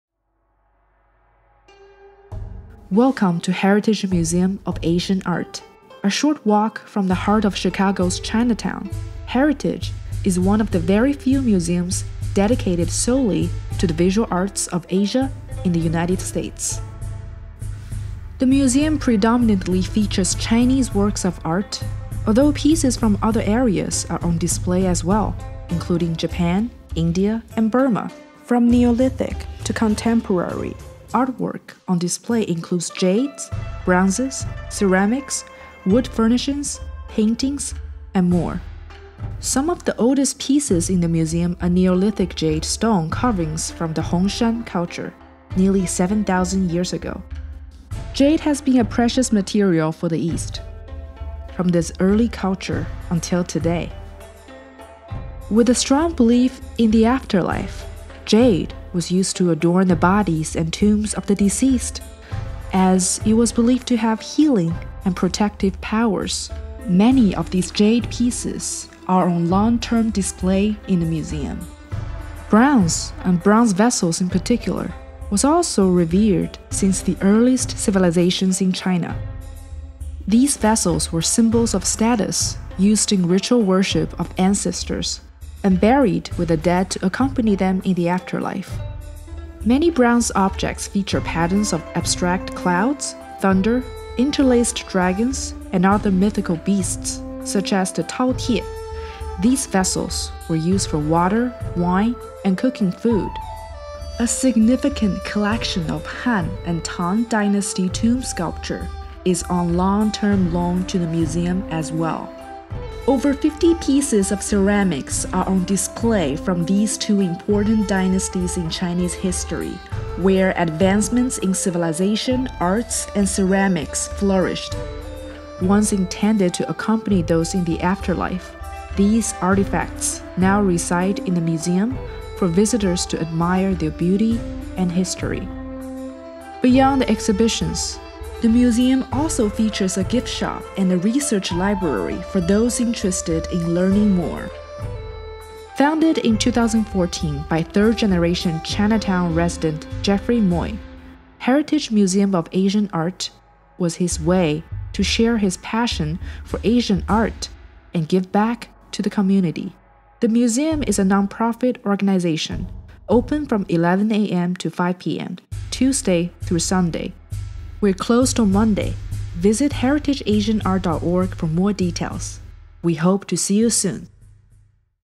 Heritage Museum of Asian Art Introduction
A brief introduction to Heritage Museum of Asian Art
Located in Chicago's Chinatown, Heritage is one of very few museums in the United States dedicated to the arts of Asia.

Heritage Museum of Asian Art
218 W 26th Street

Open 11am - 5pm Tuesday through Sunday
Closed Monday

Produced by:
SparkRaw Media